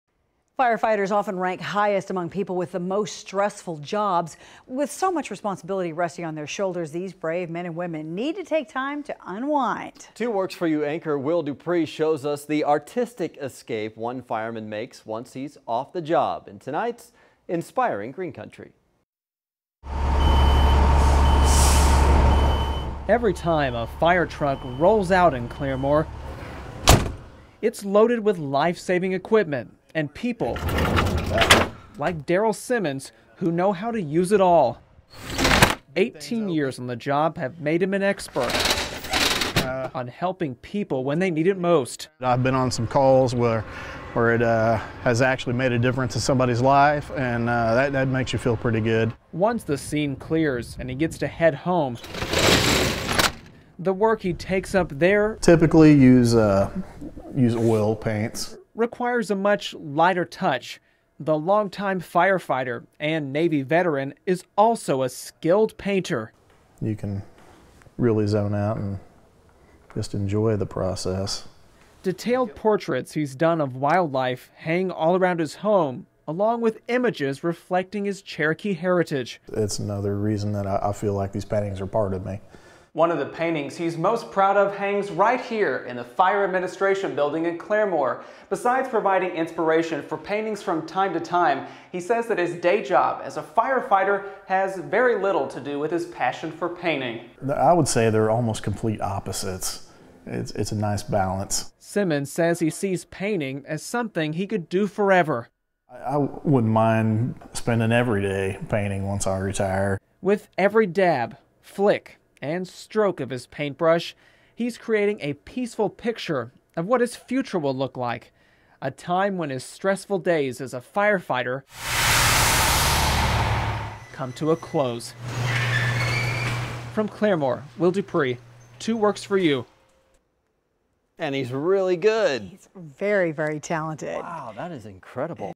Claremore firefighter finds relief from job
Took up painting as a hobby more than 10 years ago